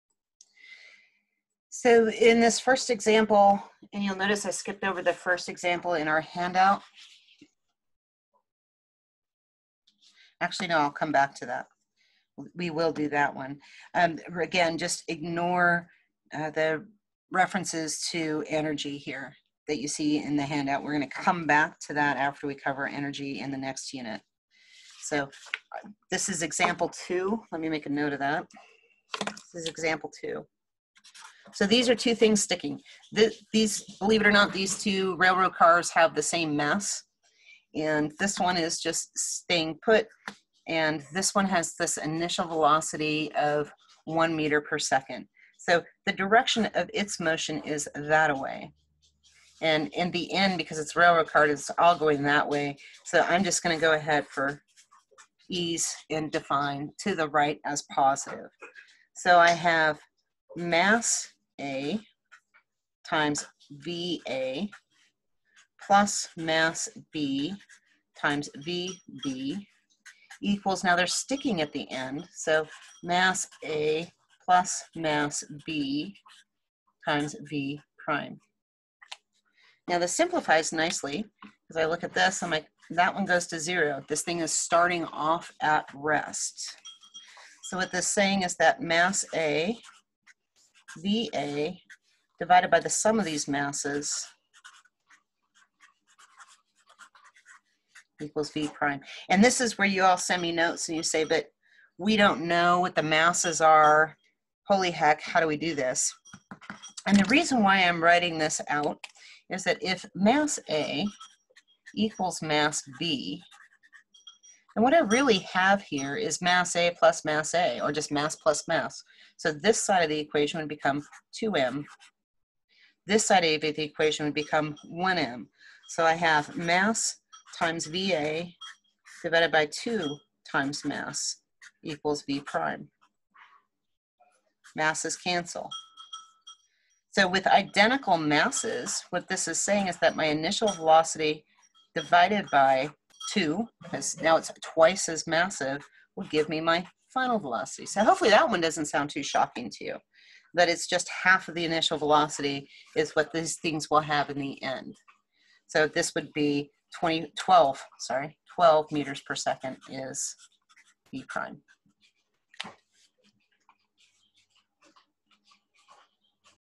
An inelastic collision determine the final velocity
An inelastic collision between two railroad cars.  Given the initial velocity of one, what is the final velocity of both?